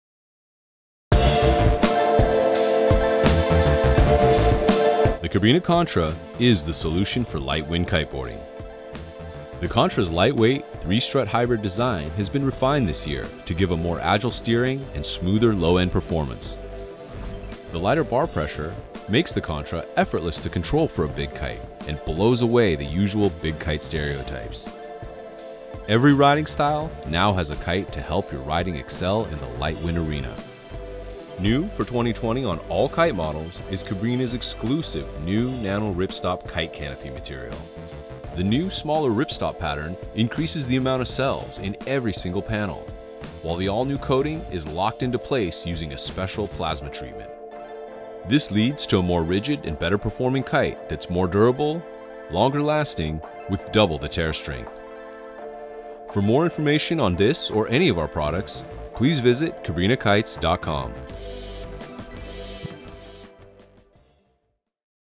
2020 Cabrinha - Contra Kite Range Review
The brand new Cabrinha Contra design offers great performance in all light wind conditions.